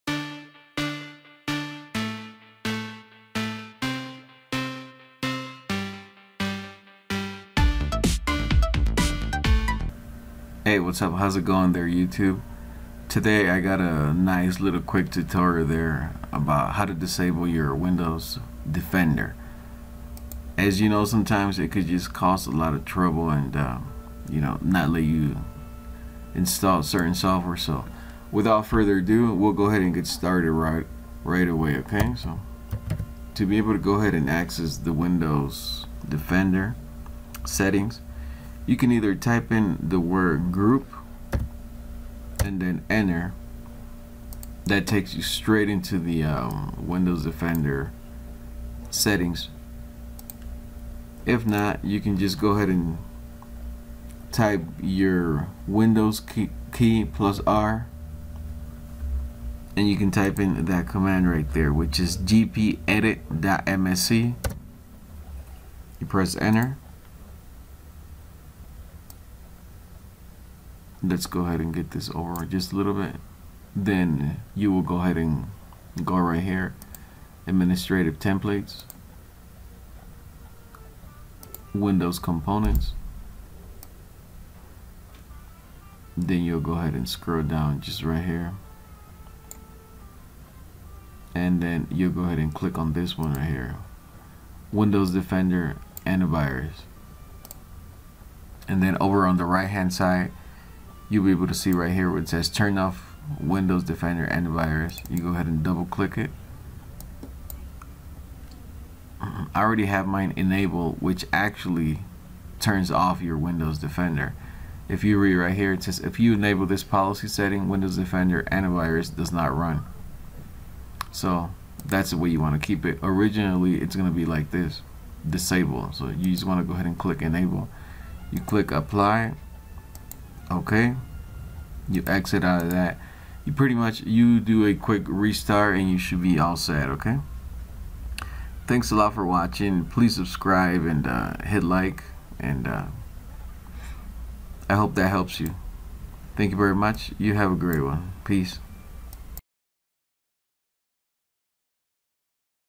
Quick simple way 😎to disable Windows Defender in Windows 10, Any version
How to Disable Windows Defender permanently in any version of Windows 10, Quick and simple.

Video made by Lewis Geek World!